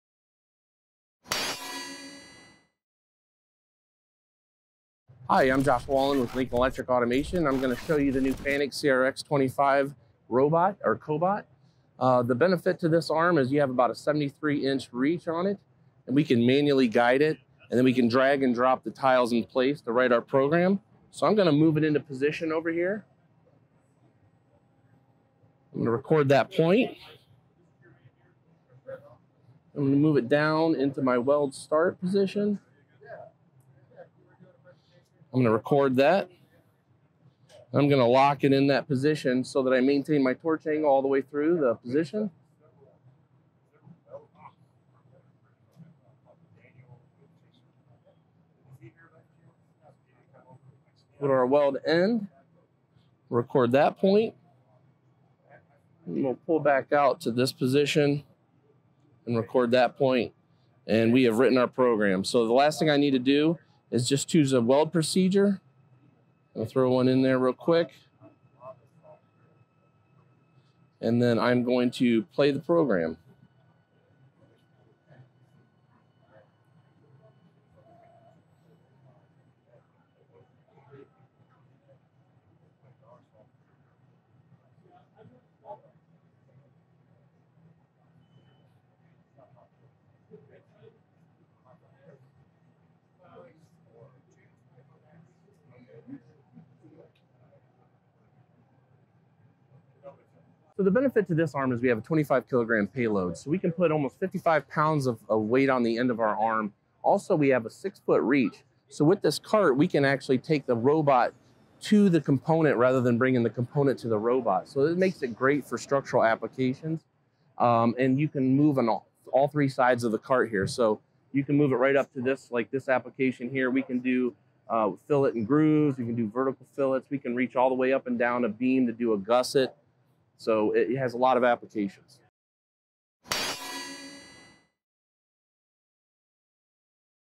Check out the NEW Cooper™ Welding Fanuc CRX- 25iA Cobot in Action
Watch the extended arm CRX-25iA Cobot be easily programmed to weld in minutes.